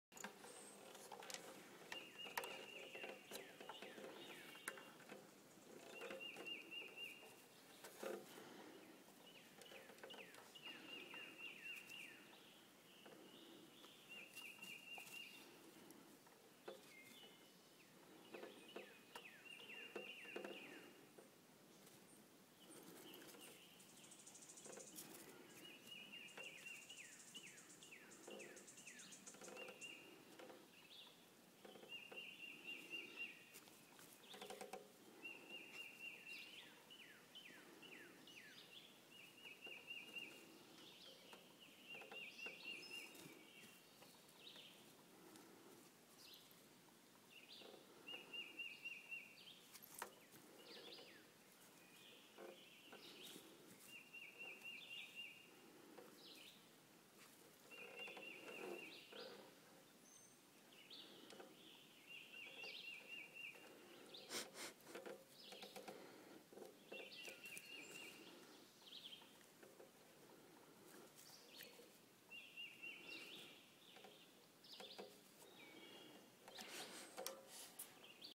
Teddy Roosevelt Terrier chasing a reflection
Jenny chases reflections from a banjo on a sunny day